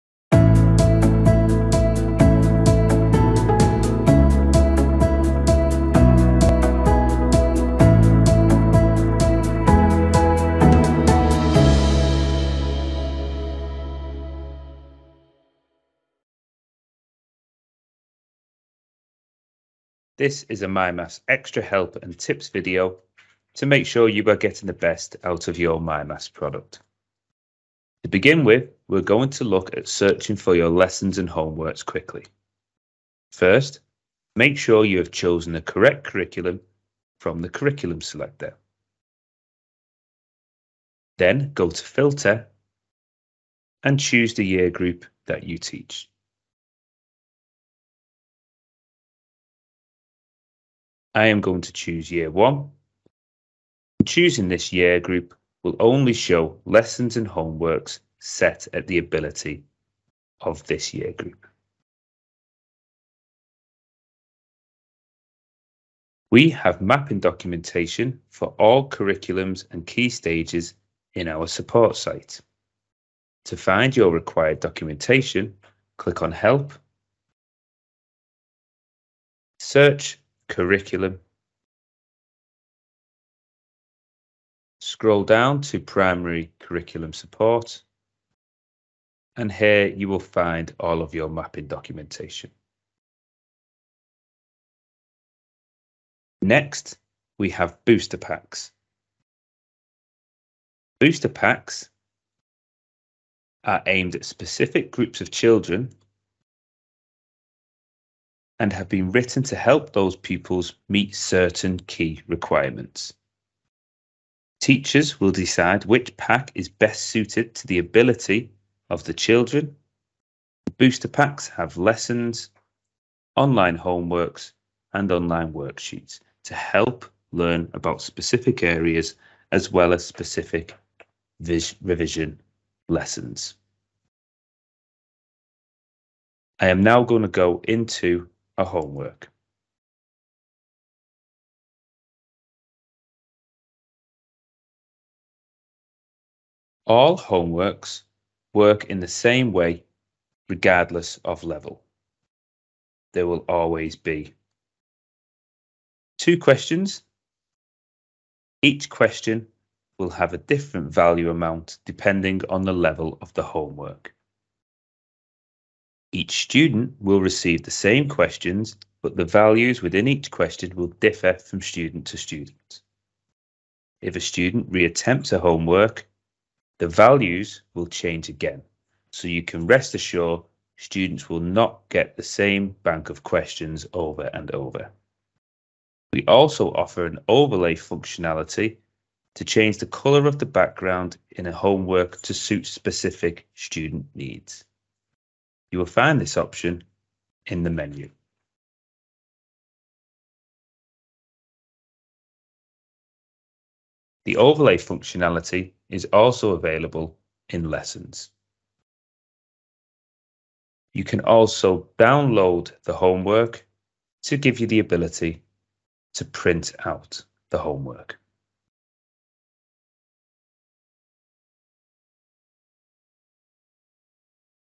Making the most of your Primary MyMaths subscription
Watch our short video on how you can get the most out of your school's Primary MyMaths subscription.